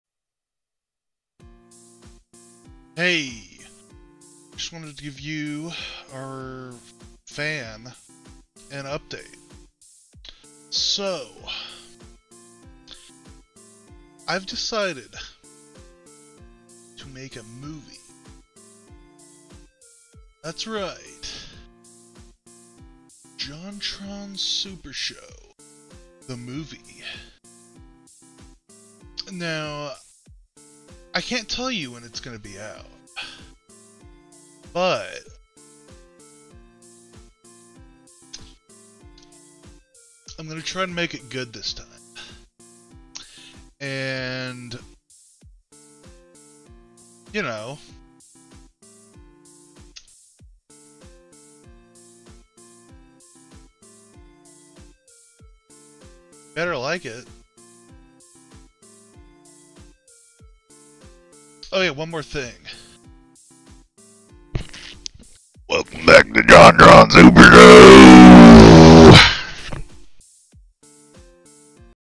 An actual update video | JohnTronSuperShow: The Movie
Welcome back! Today, JohnTron announces his future plans for the JohnTronSuperShow movie...

It's gonna be a prequel, sequel, and so much more. (If I ever finish it...(00ps) )

It's gonna be a cinematic masterpiece for the ages!!!

When this full-length movie comes out next decade, that imposter Jon "JonTron" Jafari from "JonTronShow", and his henchmen James Rofle "Angry Video Game Nerd" AVGN, Pat the NES Punk, Game Grumps (GameGrumps), and Scott the Woz will be completely blown away by this cinematic genius!!!